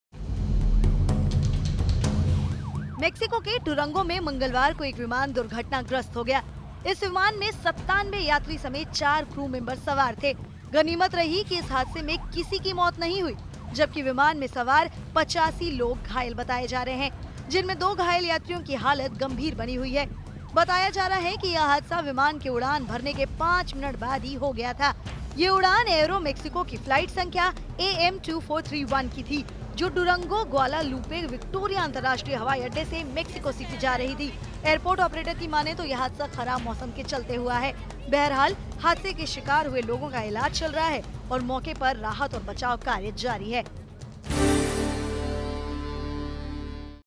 मेक्सिको में विमान क्रैश | Duniya Tak
मेक्सिको के डूरंगो में एक विमान हादसा हो गया..इस विमान 4 क्रू मेंबर समेत कुल 97 यात्री सवार थे..हादसे में 85 लोगों घायल हो गए..घायलों में 2 की हालत गंभीर बताई जा रही है ।

A plane crash took place in Durango, Mexico. The plane had 97 passengers, including 4 crew members, 85 people were injured in the tragedy..The condition of two in the rocks is being reported seriously.